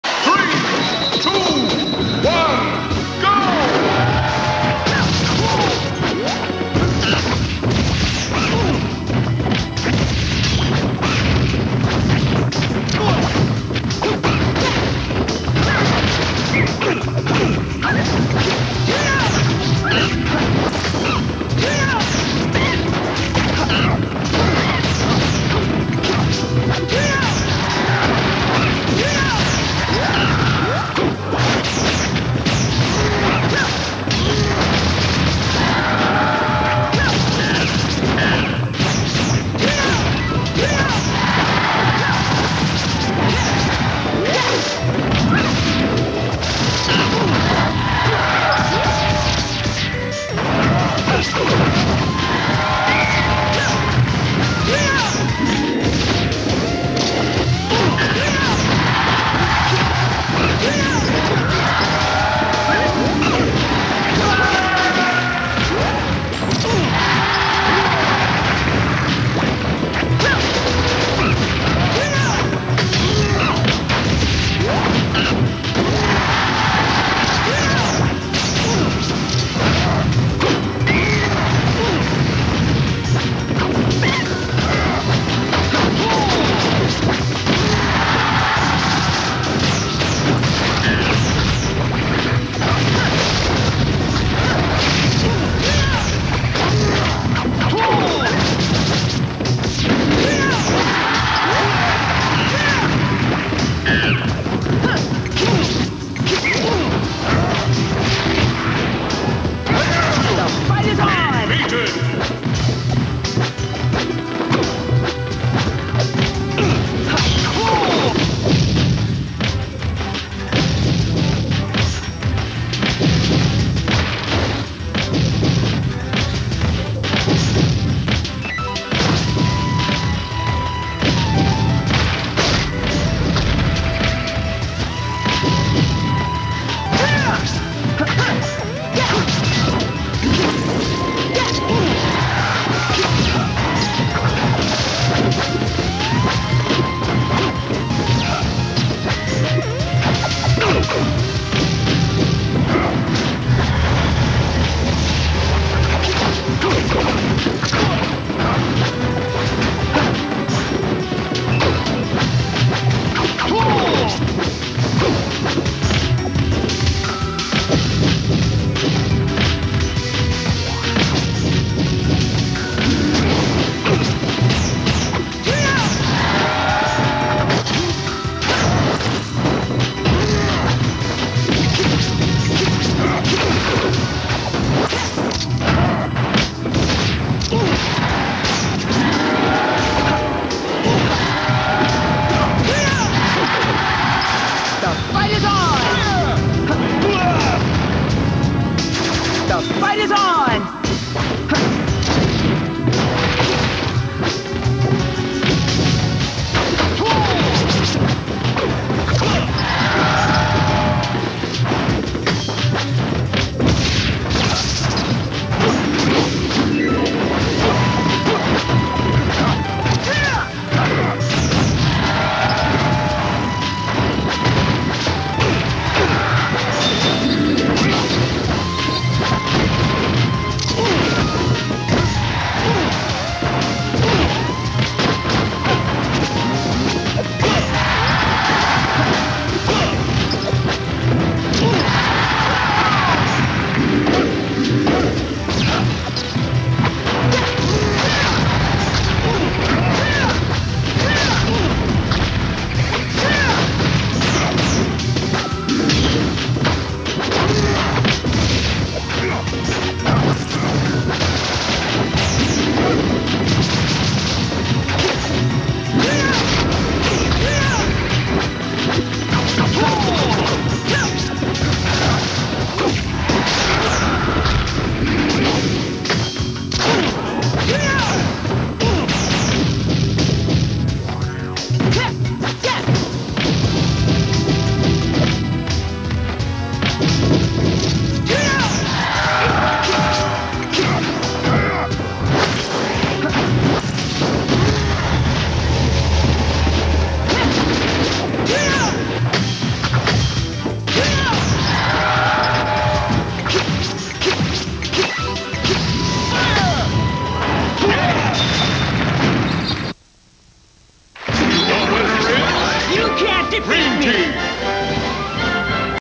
FFA R2 - Bad Boys vs Low Tier
Family Fun Arcade in Granada Hills (I don't remember when) - Bad Boys (blue) vs Low Tier (green). Round 2